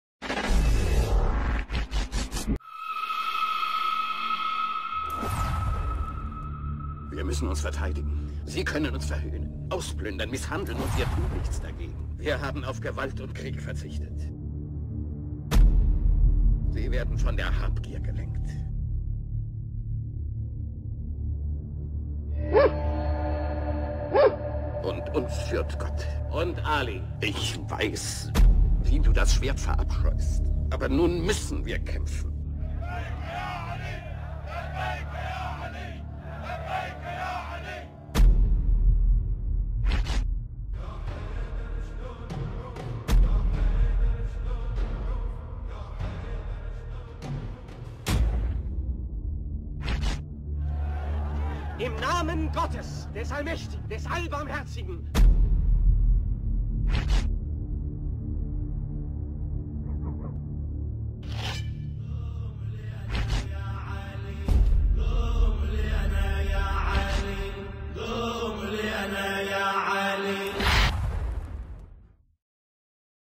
Die Kamelschlacht [Dokumentation] | Trailer ᴴᴰ
Am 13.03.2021, um 20:30 Uhr erscheint auf diesem Kanal „El Musawi“ eine Dokumentation zur Kamelschlacht, welche einer der vielen Ursprünge der heutigen Spaltung zwischen Sunniten und Schiiten, sowie des sunnitisch geprägten Terrorismus, darstellt.

In dieser Schlacht spielte auf der Gegenseite Alis, die Witwe des Propheten, 'Aisha bint Abi Bakr, eine enorme Rolle.
Die Dokumentation soll die wahre, historische Begebenheit aufzeigen und die Verdrehungen und Verzerrungen dieses historisch relevanten Ereignisses, die zugunsten der vermeintlichen „Mutter der Gläubigen“ fabriziert wurden, aus der Welt schaffen.